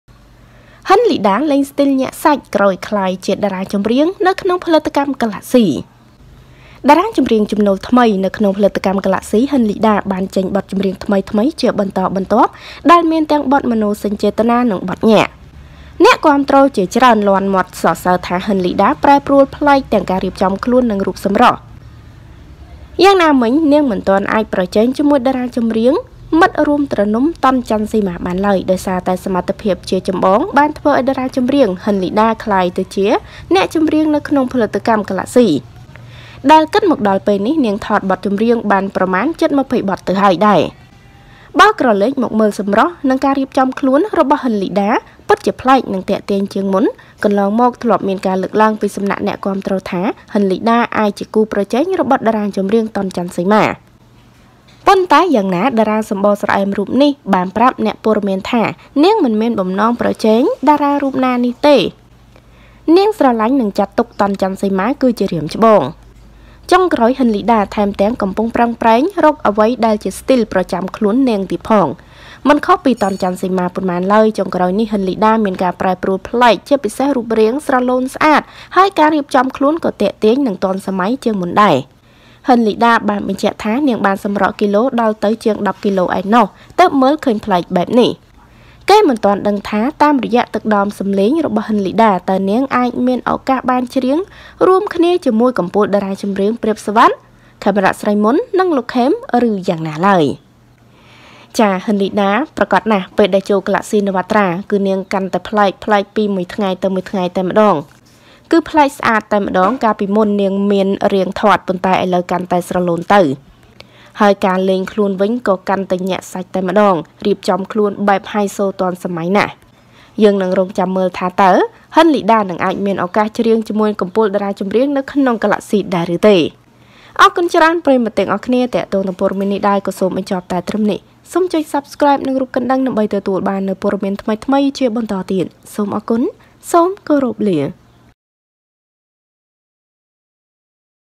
កាន់តែកាច! ហ៊ិន លីដា លេងស្ទីលញាក់សាច់ក្រោយក្លាយជាតារាចម្រៀងនៅក្នុងផលិតកម្មហ្គាឡាក់ស៊ី
សូមអធ្យាស្រ័យ

ចំពោះការមិនបានតប ខំមិនបានគ្រប់គ្នា

Ms Socheata